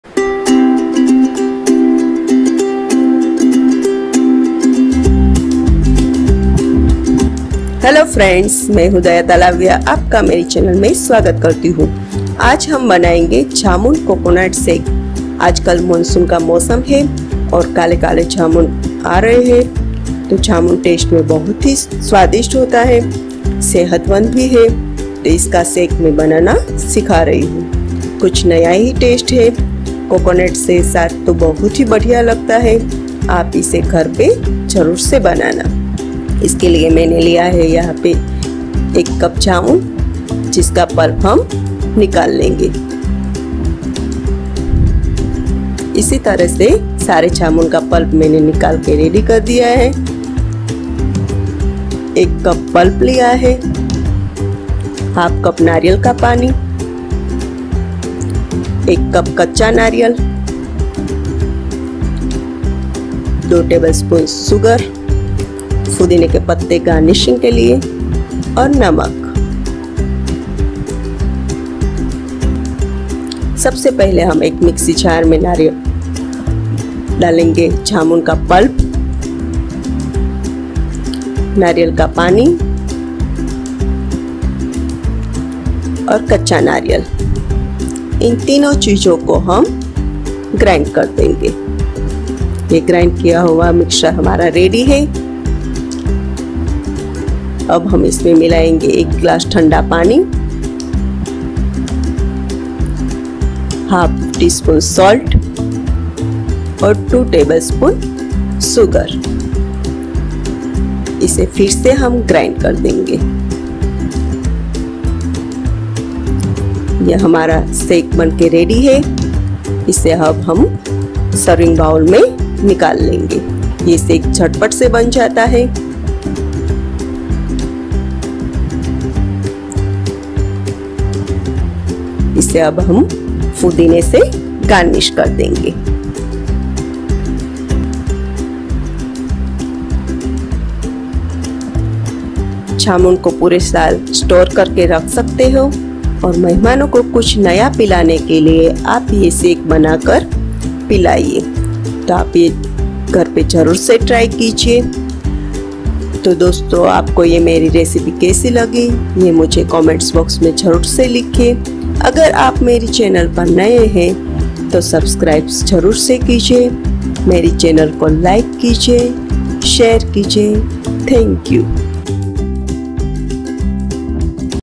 जामुन जूस | Jamun Shots | how to make jamun juice | jamunjuice | jamun coconut shake
Hello Everyone! I, Daya Talaviya. Welcome you to our channel ‘Daya's Delight Kitchen’.

Jamun coconut shake Is very healthy for Summer Season. And Cools your immune system After Consumption of this Sharbat. The consumption of Jamun Seed is healthier for Dietaries patience..

Ingredient :
Jamun
fresh coconut
fresh coconut water
Salt
Sugar
Mint leaves